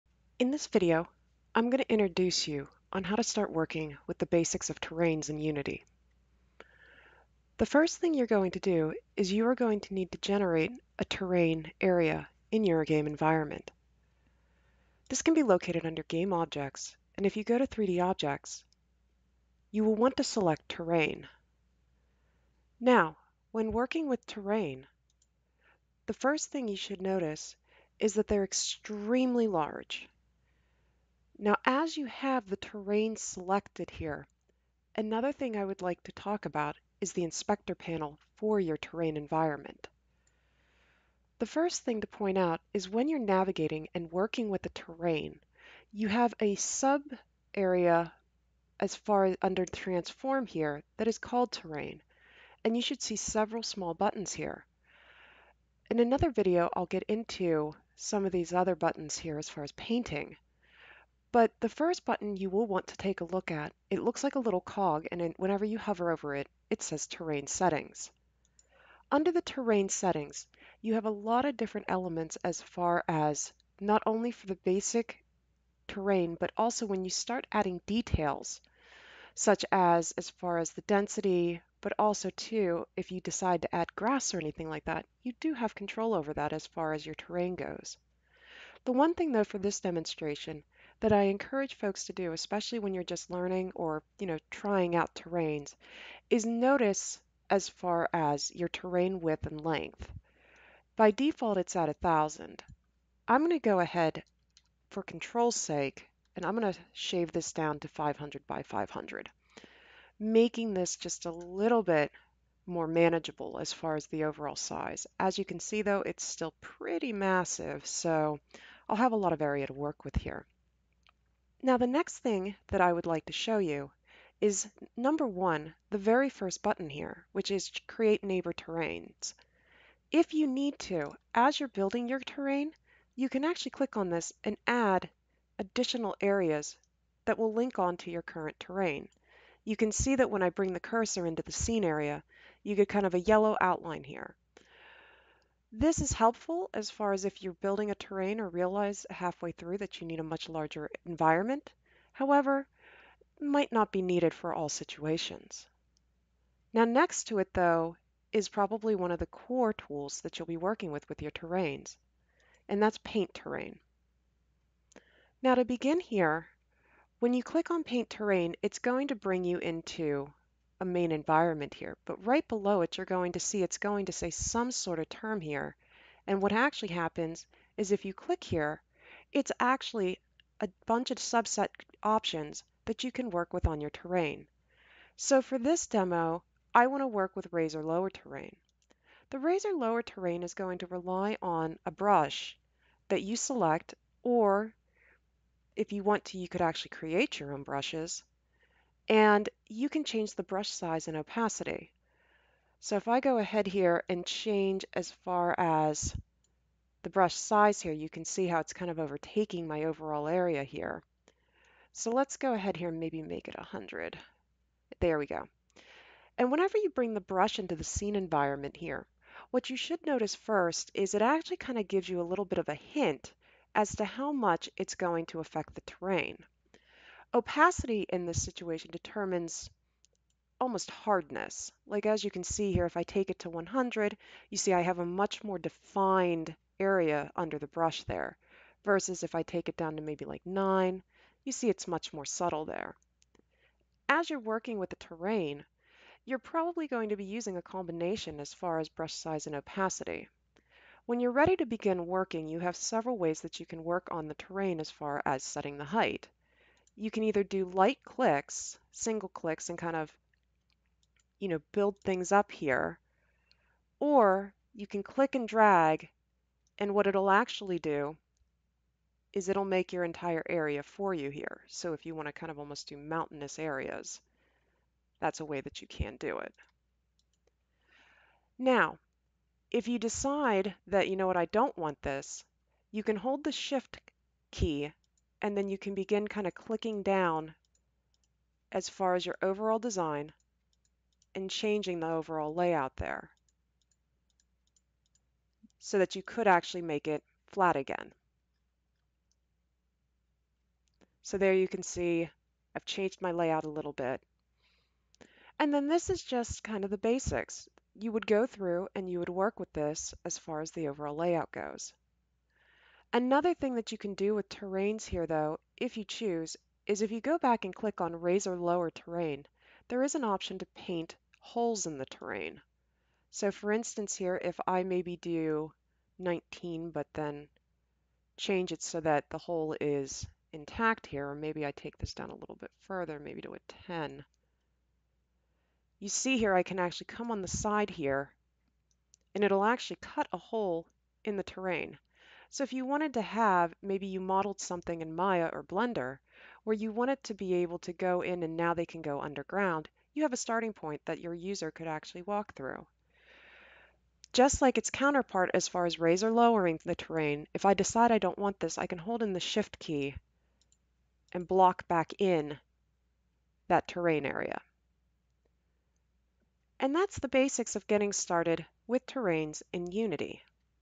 Creating a Terrain in Unity
Introduces the basics of creating a terrain and some of the options for changing the size and working with the raising and lowering tools